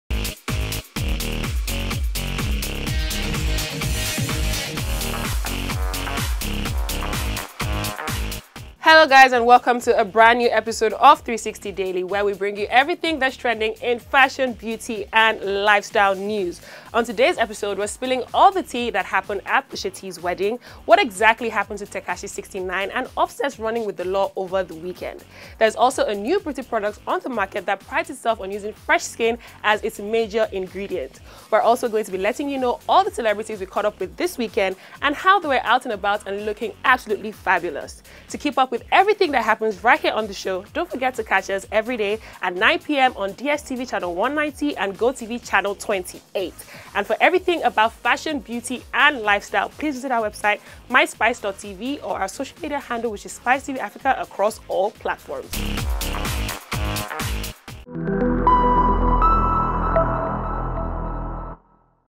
Pusha T's wedding, Beauty Product for Fresh Skin and More | 360 Daily Highlights
Pusha T's wedding, Beauty Product for Fresh Skin, XXXTentacion's collapse as well as Offset's run-in with the law and More | 360 Daily Highlights